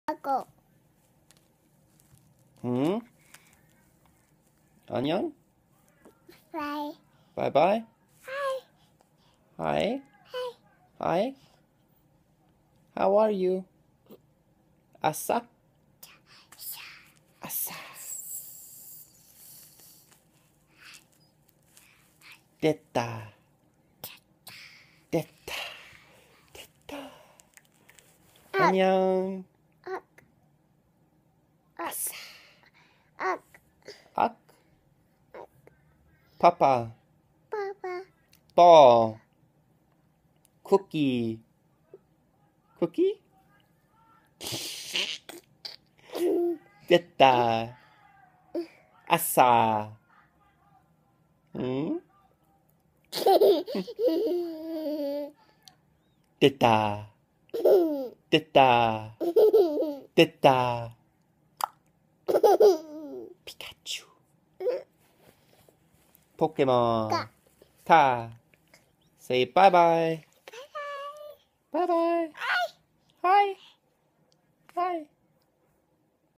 20 month old speaking Korean and English
Me and my daughter speaking together and her pulling my beard